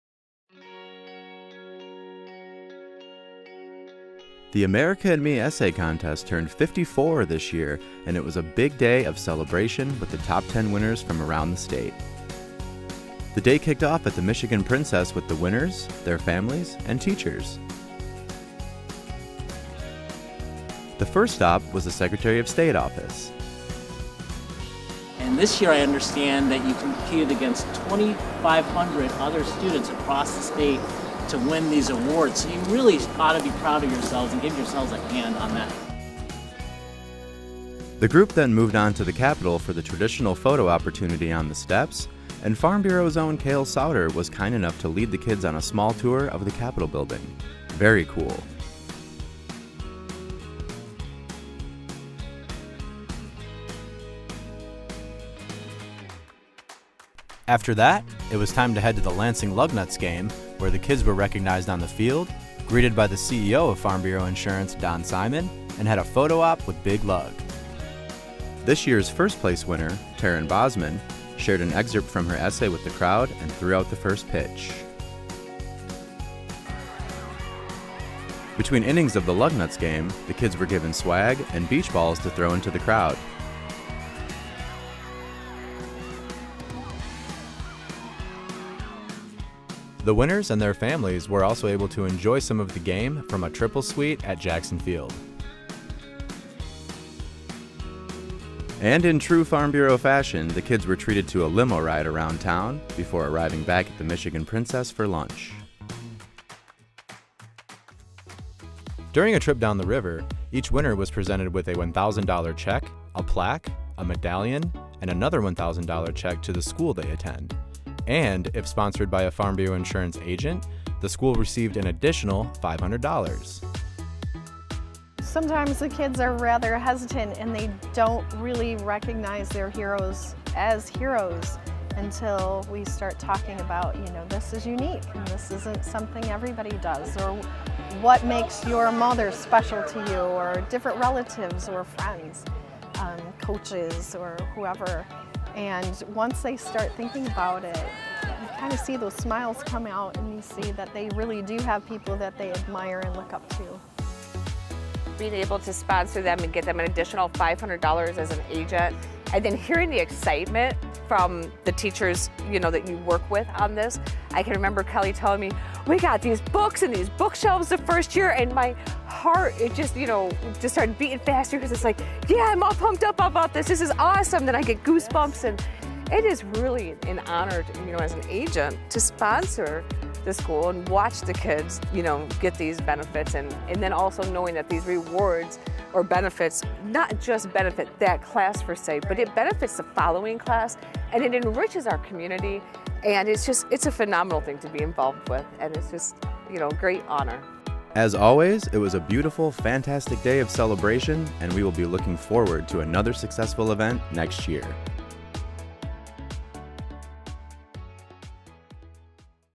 America & Me Essay Contest Awards Banquet - May 17, 2023
The Top 10 Winners from this year's America & Me Essay Contest gather in Lansing for a day of recognition and celebration!